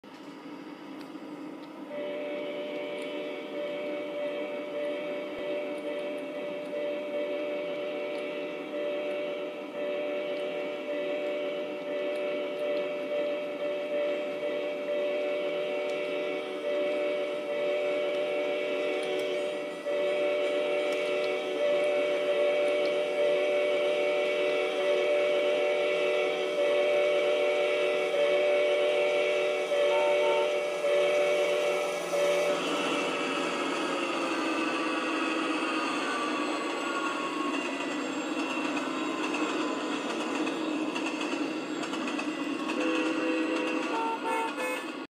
Amtrak Auto Train
First Engine:AMTK F40ph (281)
Second Engine:AMTK P32-8 (Dash 9)
Third Engine:AMTK P32-8 (Dash 9)